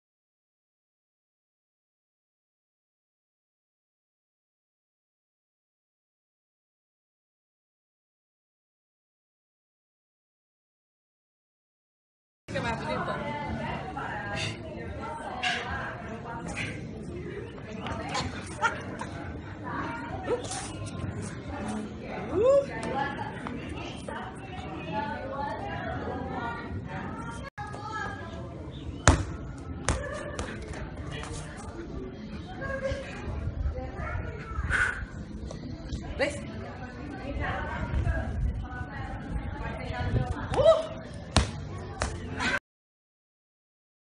SALVE O CORINTHIANS, SALVE ROBERTA MIRANDA
Roberta Miranda demonstra que  tem intimidade com a bola. Esta mulher tem muitos talentos. Brincando com o cachorrinho numa alegre e gostosa "pelada" na tarde de hoje... amo com força... Bela...menina moleca, levada da breca...
Aí pessoal do Timão, tem olheiro pra ver como também faz bonito com a bola a Rainha da Canção? Queremos Roberta Miranda na Avenida, desfilando seu talento no Carnaval Gaviões da Fiel